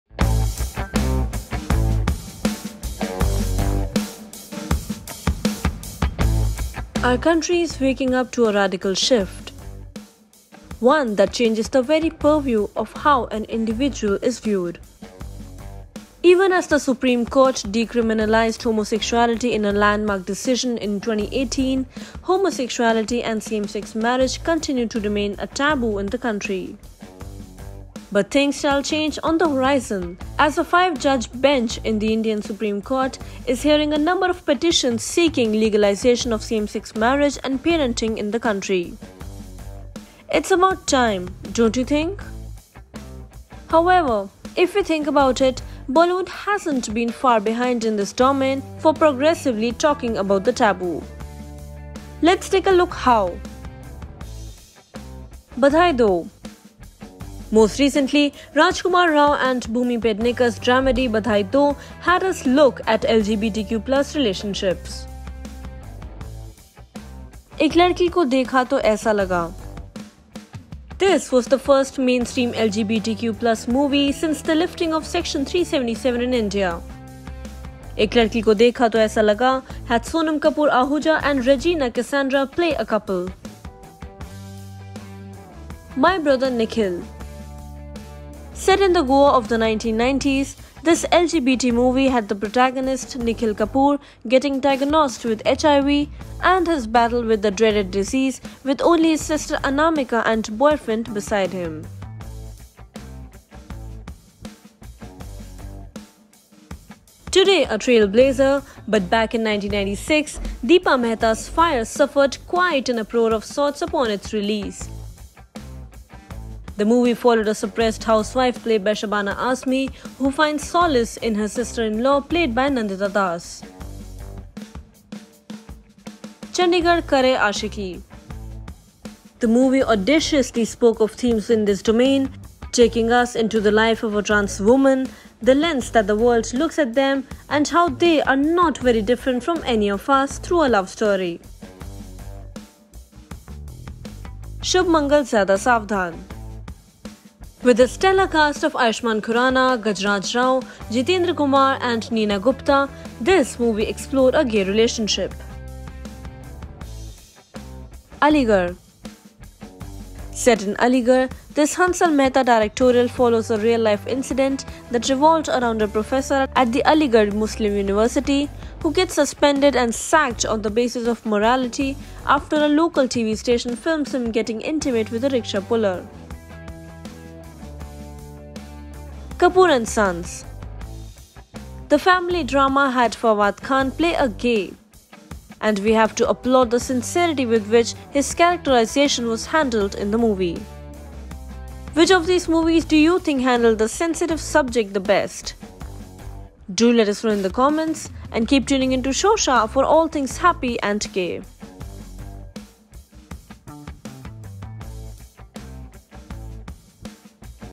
Same-Sex Marriage Case In Supreme Court: How Hindi Cinema Has Always Fought For LGBTQIA+ Rights
The Supreme Court of India is currently hearing a bunch of at least 15 petitions regarding the demand for marriage equality in India -- which, in simpler language, means validation on same-sex marriages. Hindi cinema, on its part, has always fought for LGBTQIA+ rights through its films. Watch the video where we list a few of them.